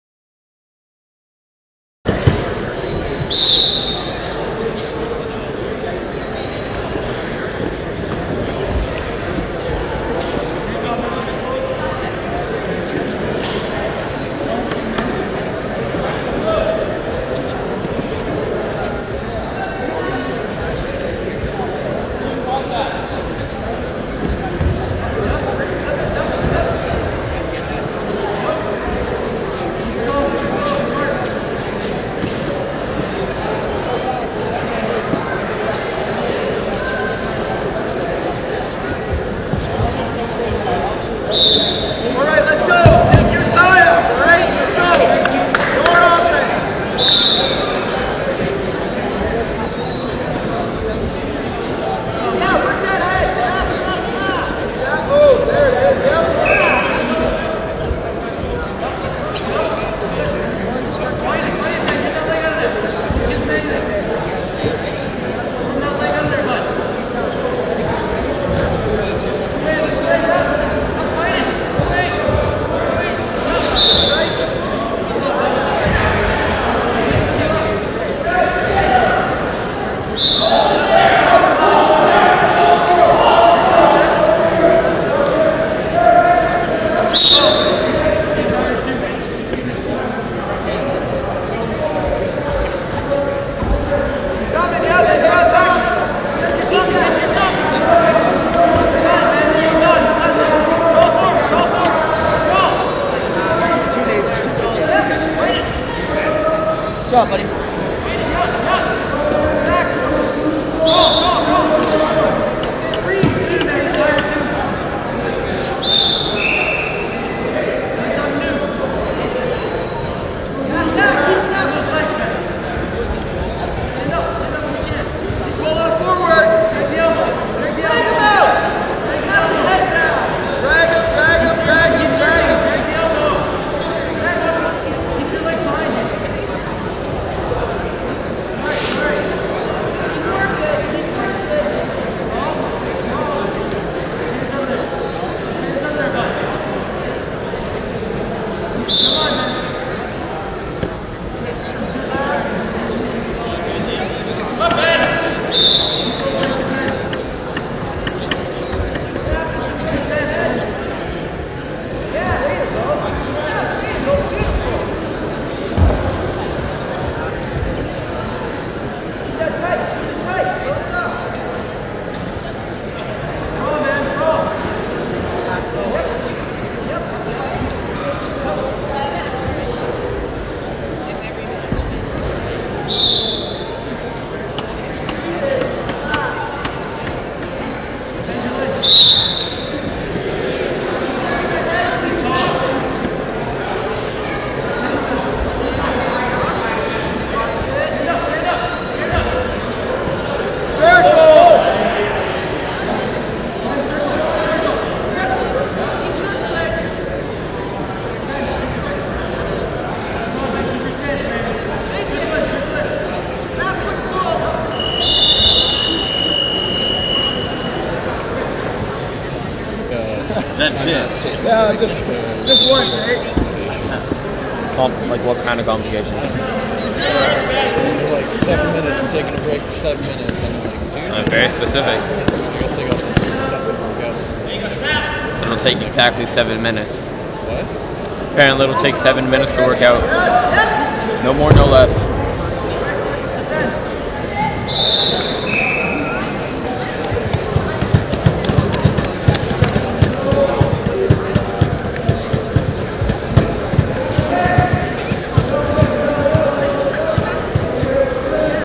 2014 Ontario Juvenile Championships: 66 kg Benjamin Vine vs. Colin Moffat
2014 Ontario Juvenile Championships 66 kg Match Between Benjamin Vine (Algoma Wrestling Club) and Colin Moffat (Dynamo Wrestling Club)